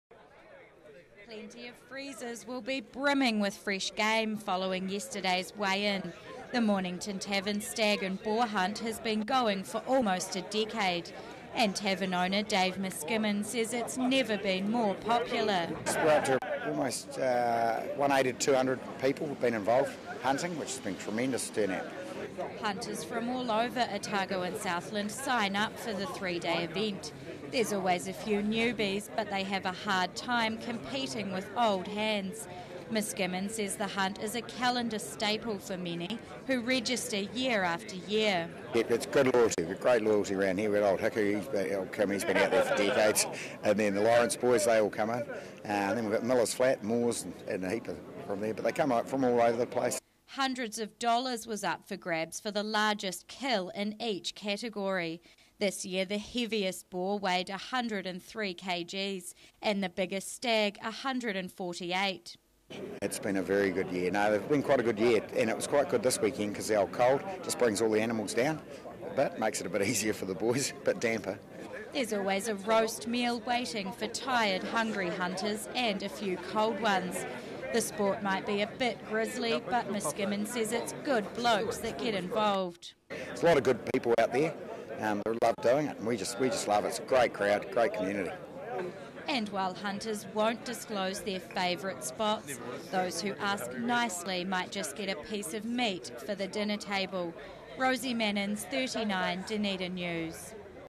A taste of country comes to town
Dunedin's a city, but every so often a taste of the country comes to town. Hundreds of hunters gathered in the suburbs yesterday, to show off their spoils from the annual Mornington Tavern Stag and Boar Hunt. It's one of the largest hunting competitions in the region, and by the looks of things participants won't be going hungry. (Warning - this story contains images that may offend.)